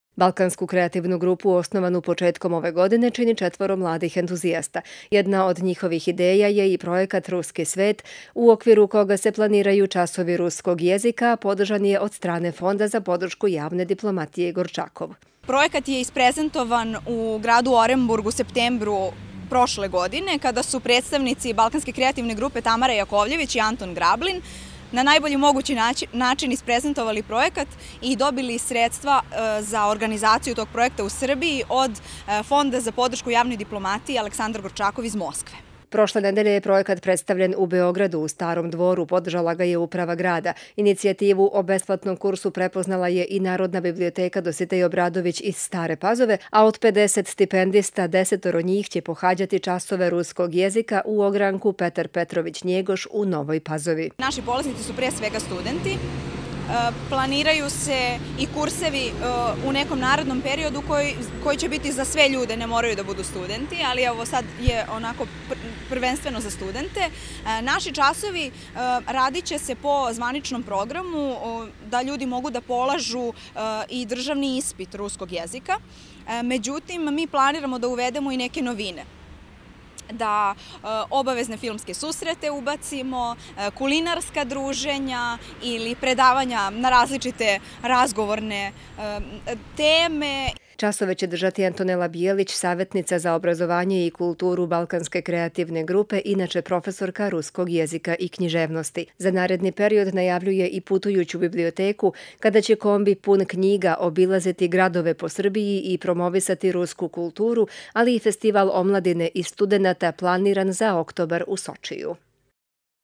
Balkanska kreativna grupa
Najveći svetski Festival omladine i studenata koji se održava u oktobru 2017.godine u Sočiju prezentovan je prošle nedelje u svečanoj sali Starog dvora.  Tom prilikom su   dodeljene  stipendije za besplatno učenje ruskog jezika uz partnerstvo Balkanske kreativne grupe i Fonda za podršku javne diplomatije Gorčakova.  Otvoreno je  i odeljenje od 10 polaznika škole ruskog jezika u Novoj Pazovi.